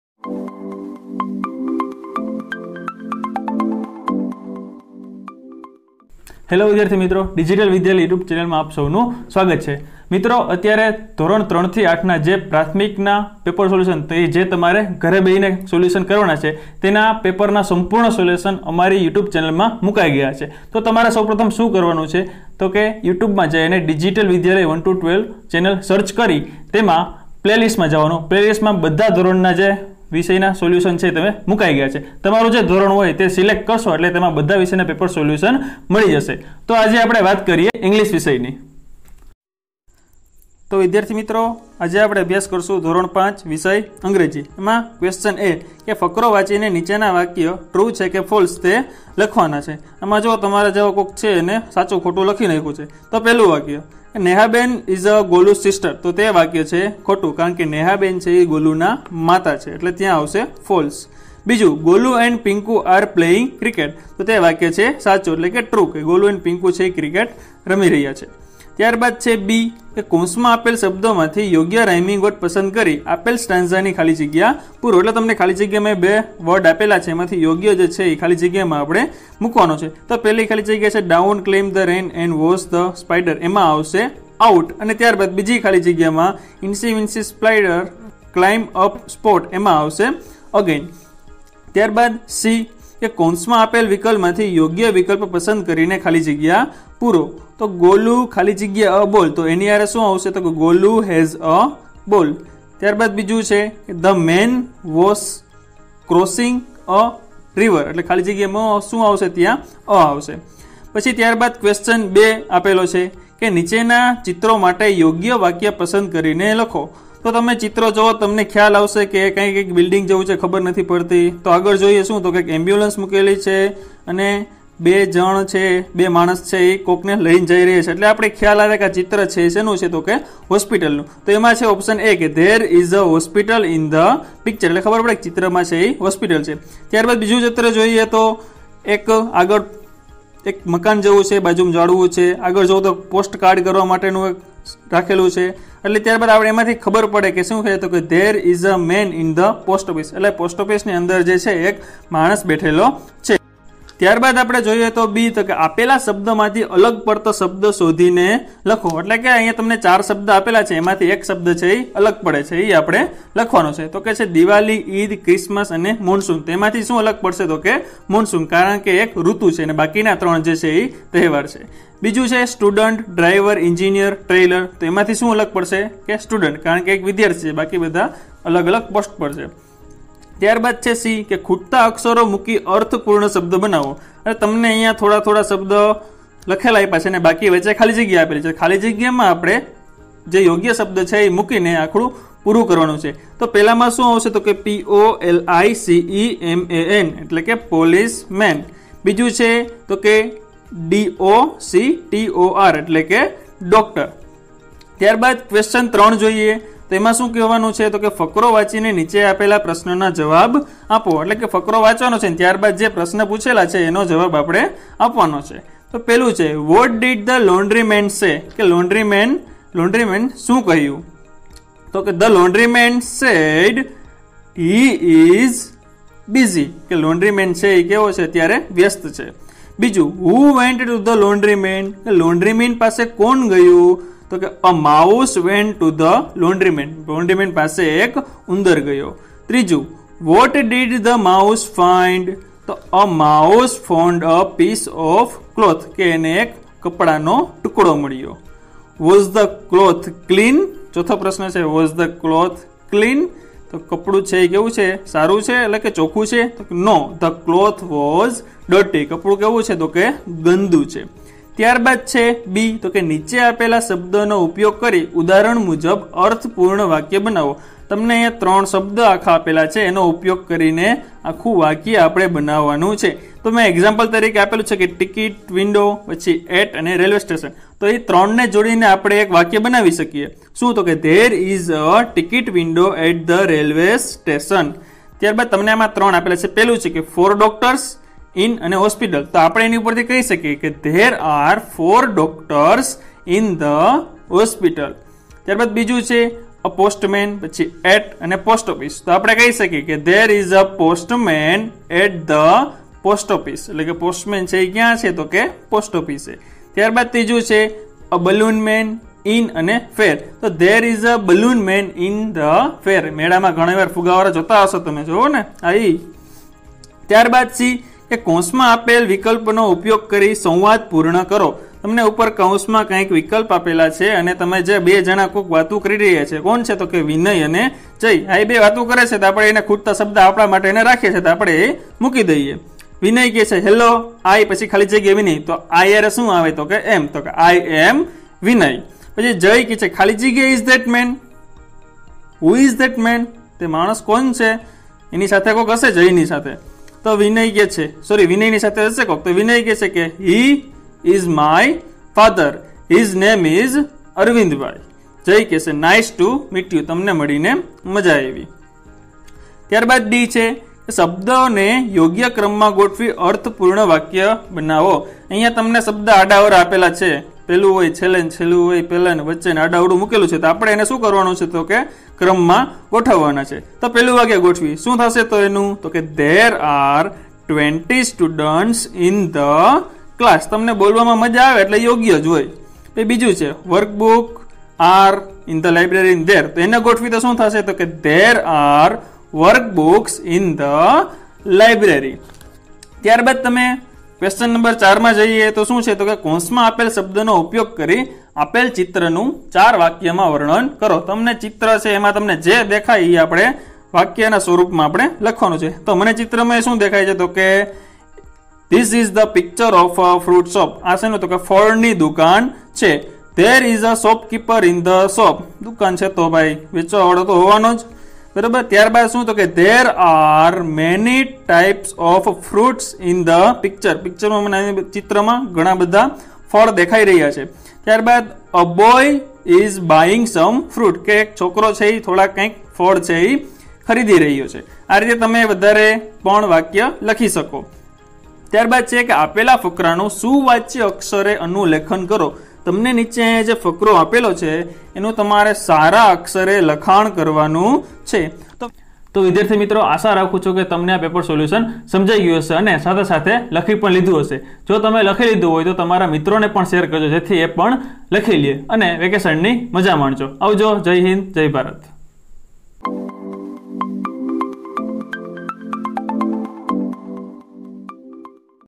ધોરણ 5 અંગ્રેજી પેપર સોલ્યુશન 2020 | Std 5 english paper solution | dhoran 5 angreji paper solution
ઘરે બેસીને લખવાના પેપર નું સોલ્યુશન
વાર્ષિક પરીક્ષા 2020 ના પેપર સોલ્યુશન

ધોરણ 1 પેપર સોલ્યુશન
ધોરણ 2 પેપર સોલ્યુશન